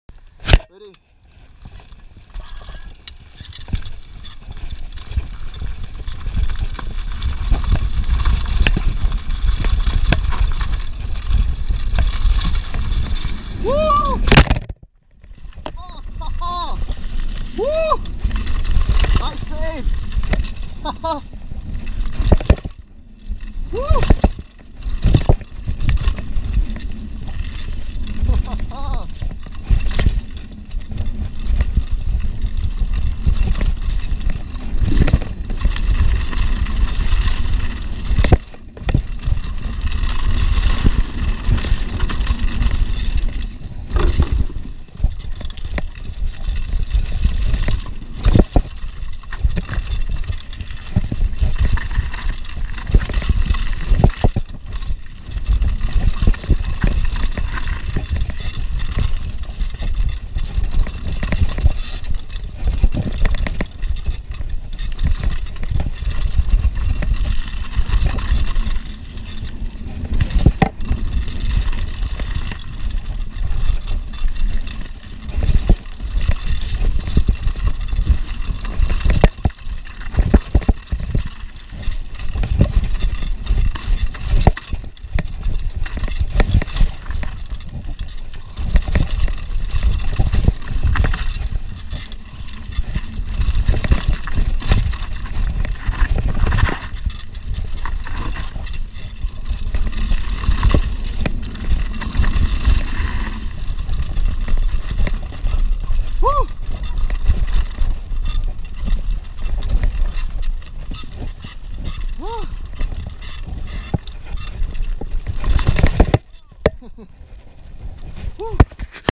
near miss on a road gap. :)
I didn't have enough speed to clear the gap and almost crashed. Saved it anyway. :)